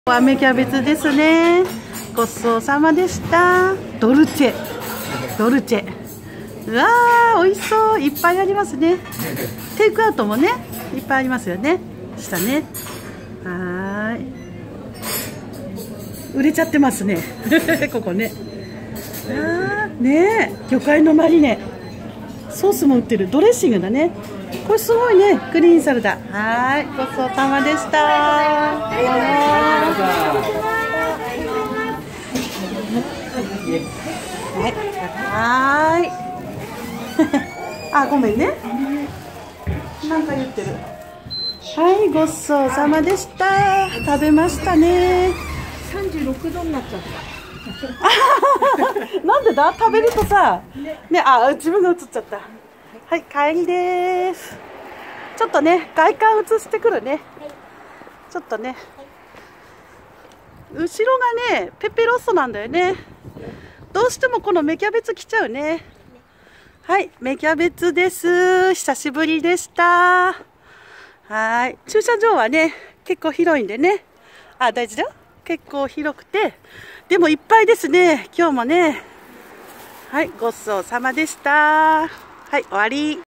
芽キャベツ🍝ちょっと店内の様子💡いろいろ美味しそうな食べ物🍰テイクアウトが並んでます💡と外観🏚️撮影許可取りました💡ごちそう様でした😋🙏
食べ終わり、いつもお腹いーーーっぱいになってしまうのですが、スパゲッティー🍝だけでもいいかなーと思うのですが、やはり飲み物🍹も、サラダ🥗も食べたい、せっかくだからデザート🍰も食べたいってなって・・
セットを頼んでしまいます🎵お腹パンパンです😆❗たまーーにだからいいかな⁉️